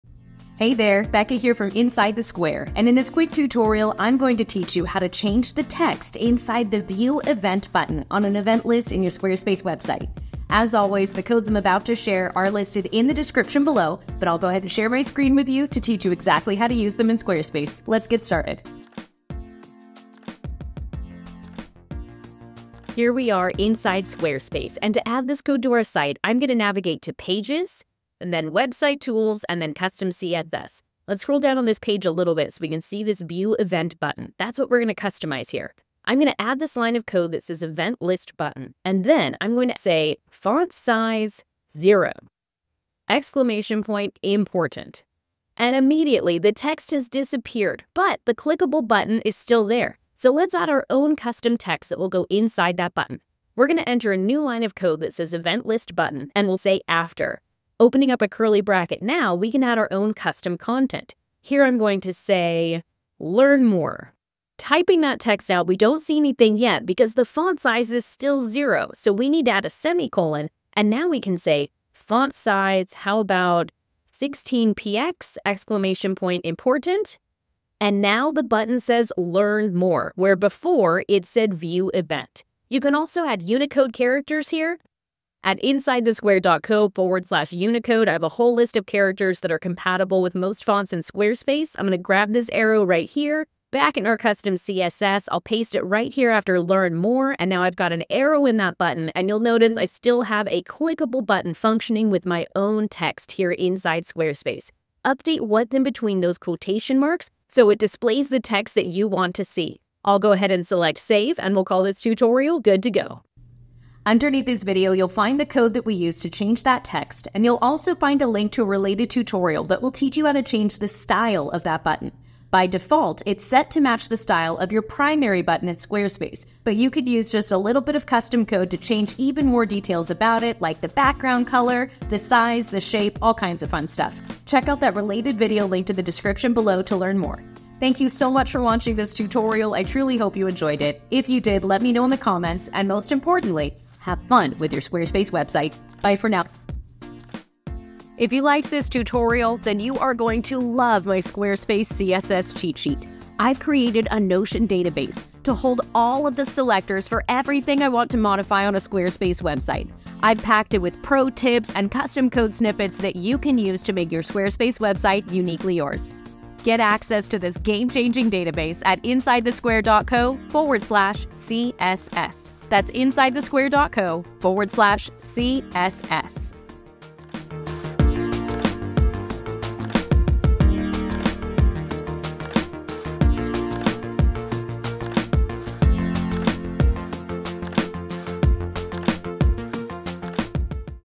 How to Change "View Event" Button Text on a Squarespace Event List Page
In this tutorial, you’ll learn how to use custom code to changing the default "View Event" button text to something unique that matches your site’s branding and voice. Squarespace doesn’t provide built-in tools to edit this button’s text, but with a simple CSS trick, you can change it to say anything you want.

In this quick and easy tutorial, I’ll guide you through every step of the process. You’ll learn how to remove the default text, add your own custom wording, and even include fun Unicode characters like arrows or icons for added flair. This is a great way to personalize your event list and create a polished, professional look.

⏱️ Important Timestamps:
00:01 - Introduction: What You’ll Learn
00:31 - Accessing Custom CSS to Edit the View Event Button
00:59 - Removing Default Button Text with CSS
01:32 - Adding Custom Text and Unicode Characters to the Button
02:00 - Related Tutorial: Changing the Style of the View Event Button
02:26 - Squarespace CSS Cheat Sheet Overview & Closing

🖥 Tutorial Code
/* event button code from insidethesquare.co */
.eventlist-button {
  font-size:0!important
.eventlist-button:after{
  font-size:16px!important
Need help with customizing your site? Here are my current support options